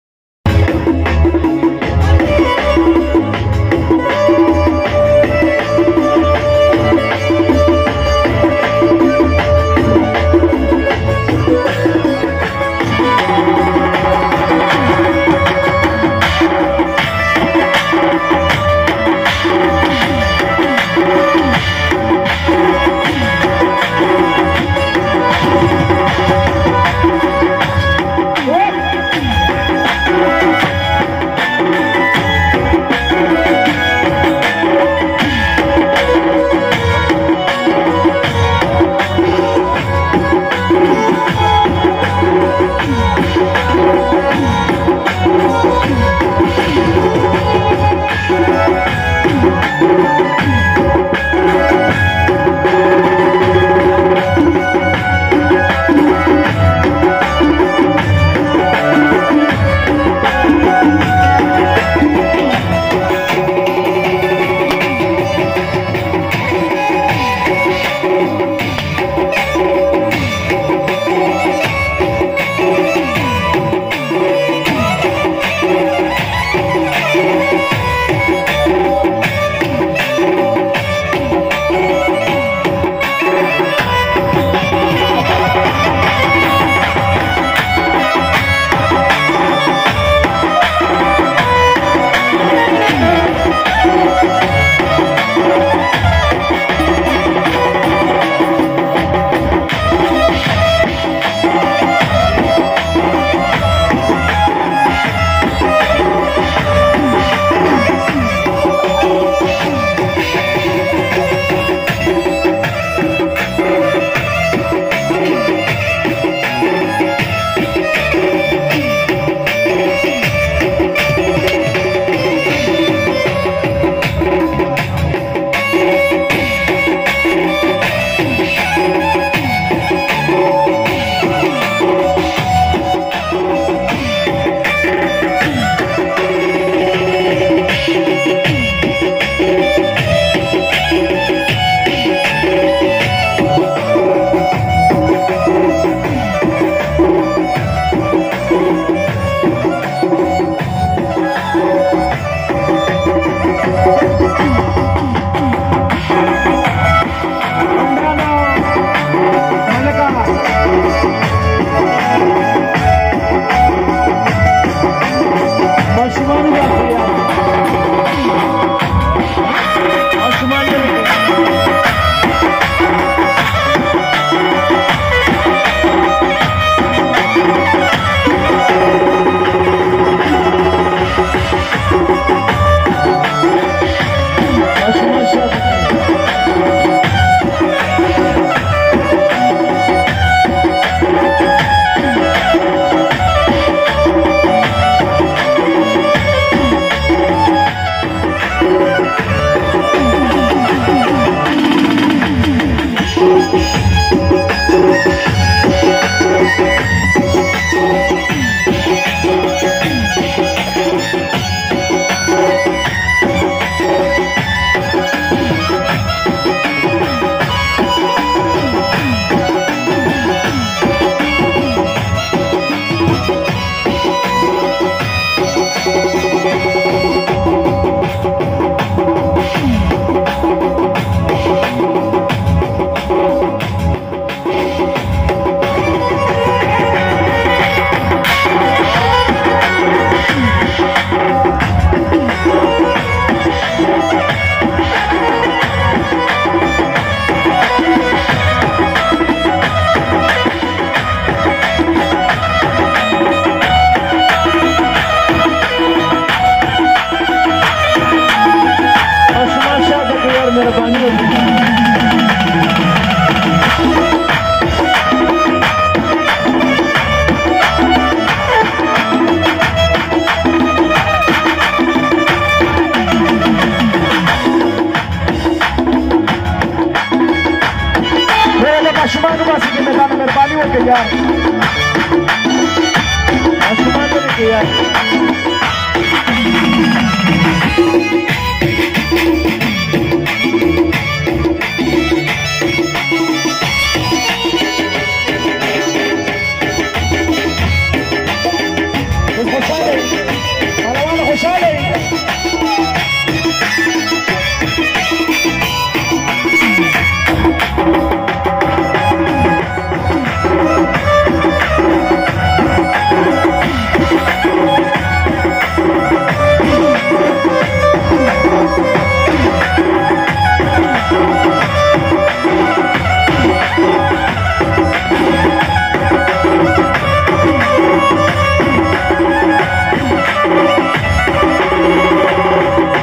Afghan Keyboard Parda Awal 2021 | Abshari pashto mast saaz
QATAGHANI pashto songpashto mast saazpashto new song 2021keyboard Instrumental
The song and music is recorded by the channel owner
Yousaf khan akakhail
Which is made in his friend wedding for full enjoyment and zafar supari was also in the wedding the wedding was not high profile but the guests were very excited and Vip so
Keep supporting and keep watching

THANKS 🥰